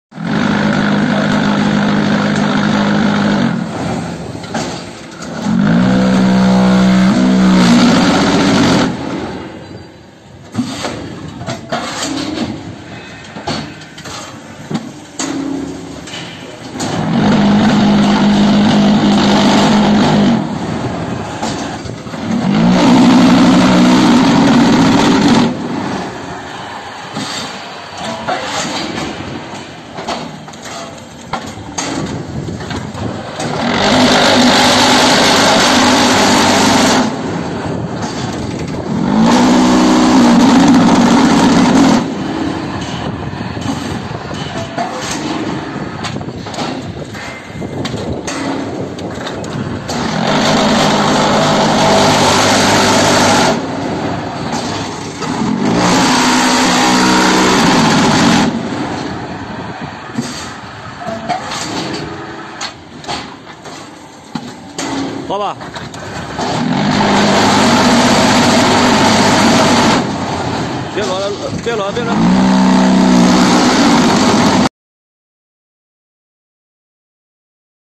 QT4 15 automatic hollow block machine knock block electronic control WhatsApp +86 18315754985
This is qt4-15 fully automatic PLC frequency converstion control block making machine, running by Three phase electricity, indutrial voltage. It produce knock block, hollow block, solid brick, interlocking brick, paver brick etc. by changing mold.